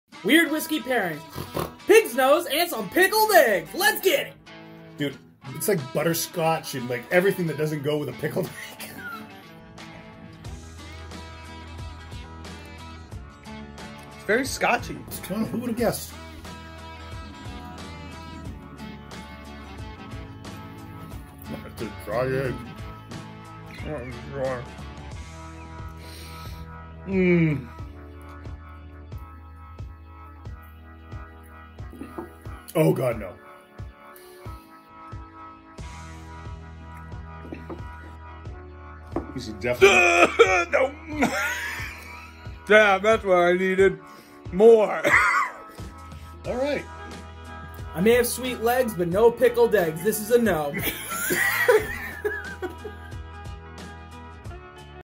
Pickled eggs and Pig's Nose Blended Scotch (Weird Whiskey Pairing)
We try Pig's Nose Blended Scotch Whisky and Pickled Eggs for this weird whiskey pairing. Is it a go or a no?

So " Are you Ready and Abel to Whiskey"? If you are, enjoy the show!

Do you like the Chug Jug Can? Go to Facebook and check out-